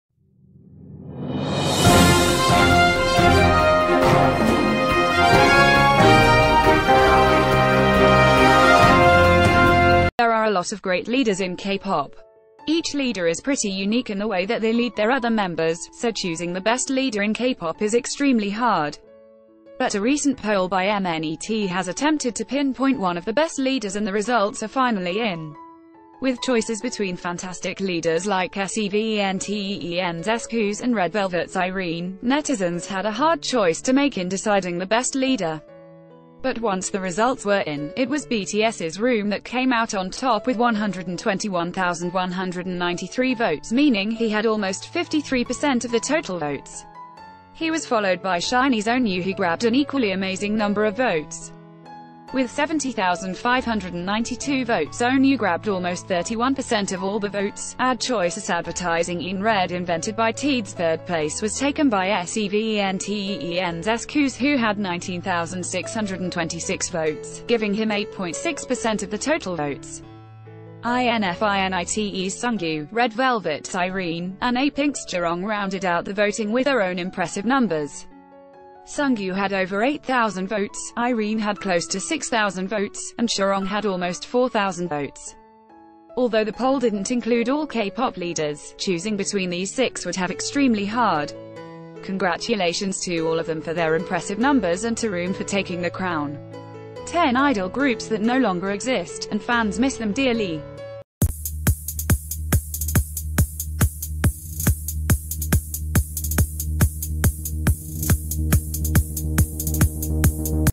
+200,000 Koreans Voted And BTS’s RM Was Declared The Best Leader In K-Pop
There are a lot of great leaders in K-Pop. Each leader is pretty unique in the way that they lead th...